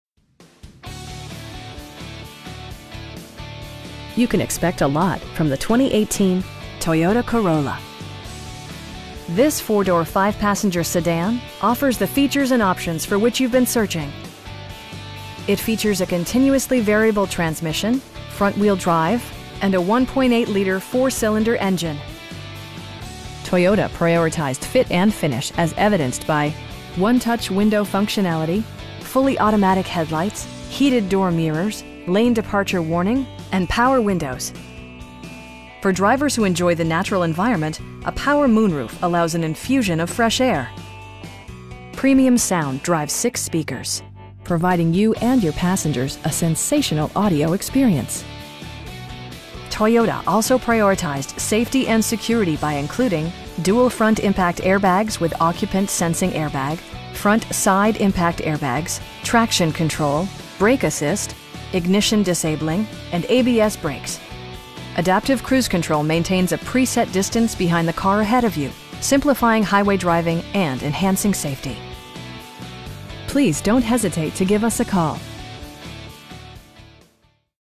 2018 Toyota Corolla XLE in Petaluma, CA 94952
Hansel Toyota

You can expect a lot from the 2018 Toyota Corolla. This 4 door, 5 passenger sedan offers the features and options for which you've been searching. It features a continuously variable transmission, front-wheel drive, and a 1.8 liter 4 cylinder engine. Toyota prioritized fit and finish as evidenced by: 1-touch window functionality, air conditioning, fully automatic headlights, tilt steering wheel, heated door mirrors, lane departure warning, and power windows. For drivers who enjoy the natural environment, a power moon roof allows an infusion of fresh air. Premium sound drives 6 speakers, providing you and your passengers a sensational audio experience. Toyota also prioritized safety and security by including: dual front impact airbags with occupant sensing airbag, front side impact airbags, traction control, brake assist, ignition disabling, and ABS brakes. Adaptive cruise control maintains a pre set distance behind the car ahead of you, simplifying highway driving and enhancing safety. We have the vehicle you've been searching for at a price you can afford. Please don't hesitate to give us a call.